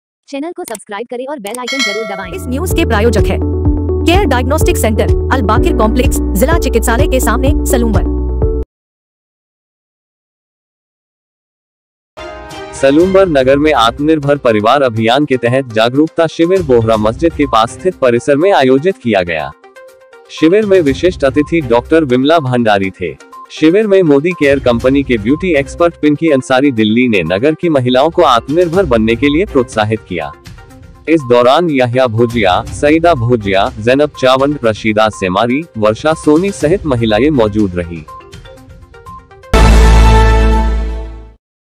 सलूम्बर में आत्मनिर्भर परिवार अभियान के तहत जागरूकता शिविर आयोजित
न्यूज़ salumber
mewad kranti news
mewar kranti
mewar kranti news
mewad kranti
मेवाड़ क्रांति
मेवाड़ क्रांति न्यूज़